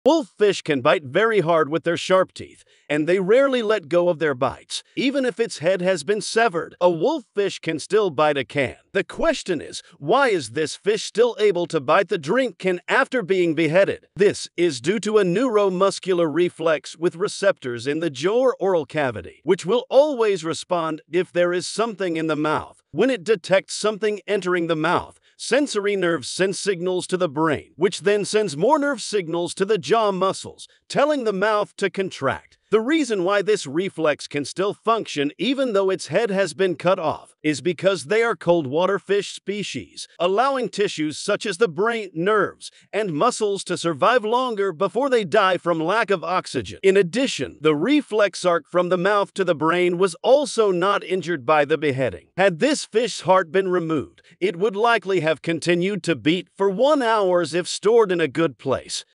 The Reason Wolf Fish Can Still Bite Even After Having Their Heads Cut Off
The Reason Wolffish Can Still Bite Even After Having Their Heads Cut Off